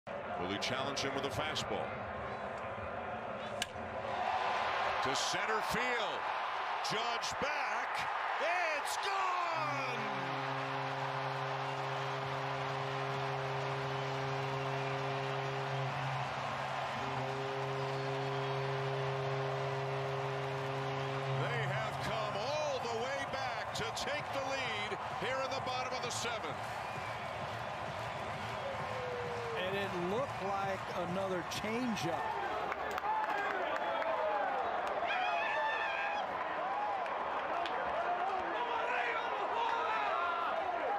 Teoscar Hernández Hits GO-AHEAD 3-Run Homer (5 Run Comeback)
Teoscar Hernández hits a clutch 3-run home run to give the Blue Jays a 10-8 lead over the Yankees in the 7th.